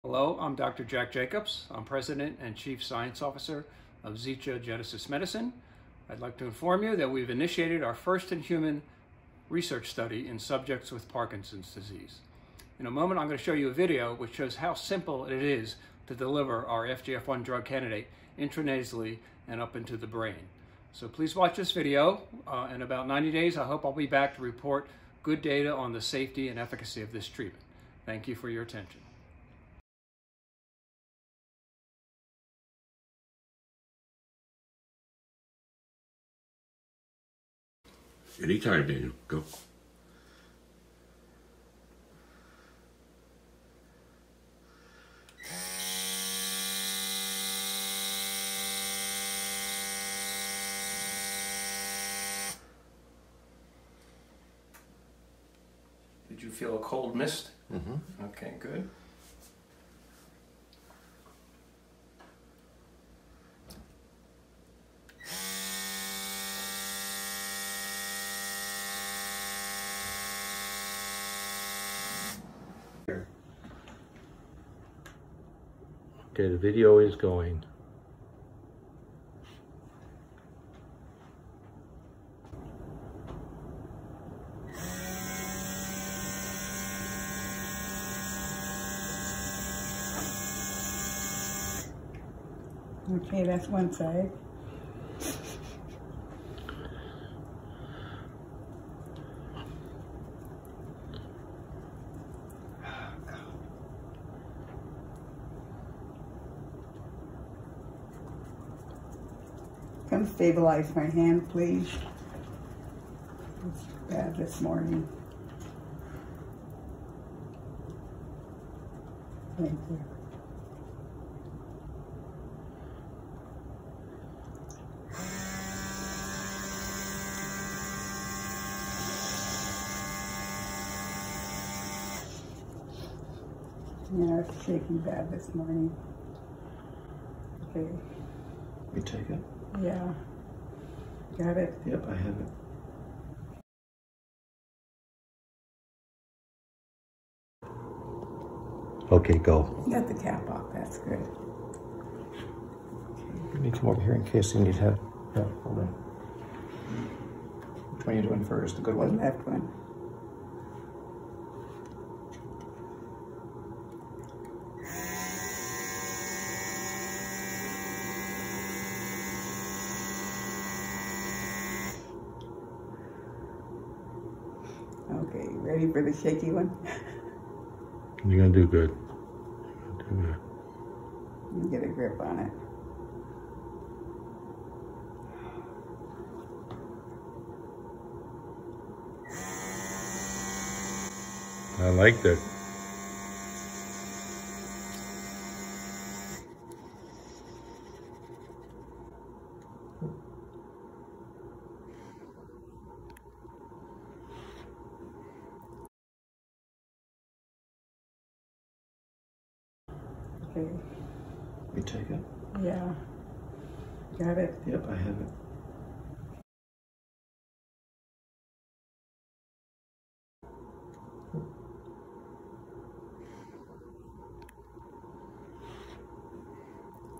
Parkinson's Disease- Medical Research Study- May 19, 2022
This video is a first look into Zhittya Genesis Medicine's Medical Research Study into testing intra-nasally delivered FGF-1 in patients with Parkinson's Disease.